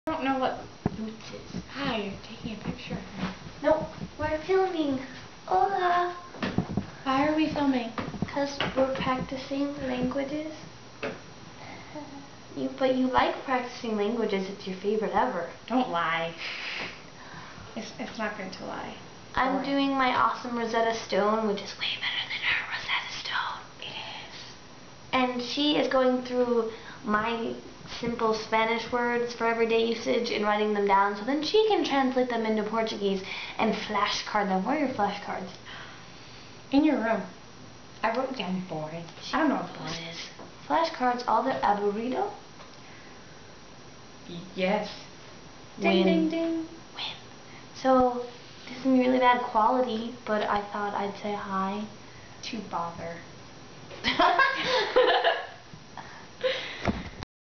1st Study Session
Paige looking up portuguese verbs and Marquie doing some rosetta stone.